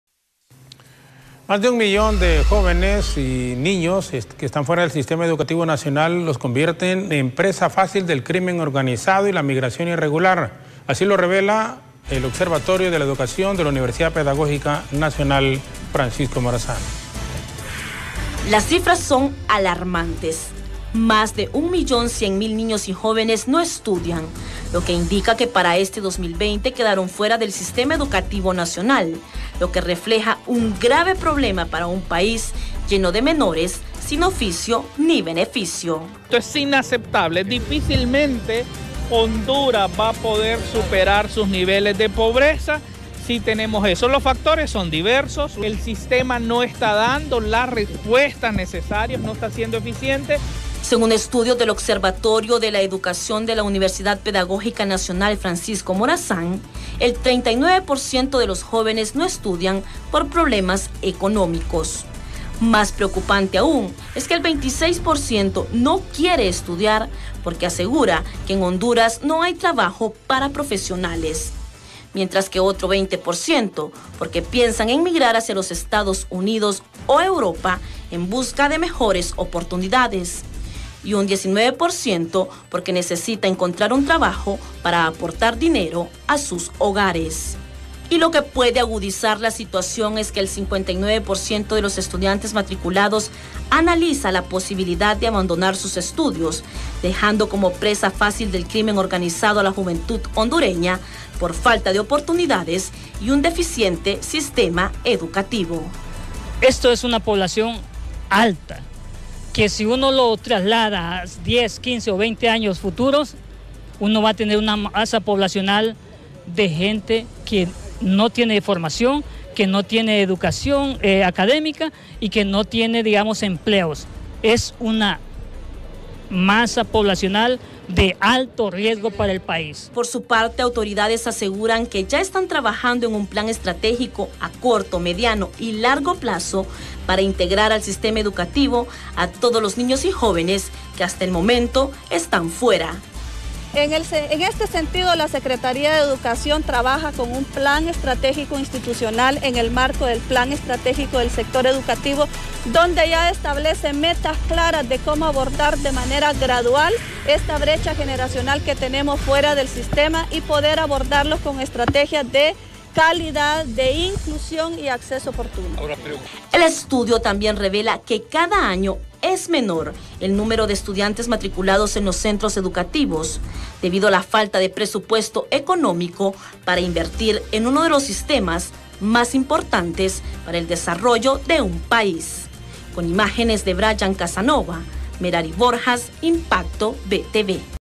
Más de un millón de menores de edad están fuera del sistema educativo nacional
Más de un millón de jóvenes y niños están fuera del sistema educativo nacional, convirtiéndolos en presa fácil del crimen organizado y la migración irregular, así lo revelo  el Observatorio de la Educación de la Universidad Pedagógica Nacional Francisco Morazán. ▂▂▂▂▂▂▂▂▂▂▂▂▂▂▂▂▂▂▂▂▂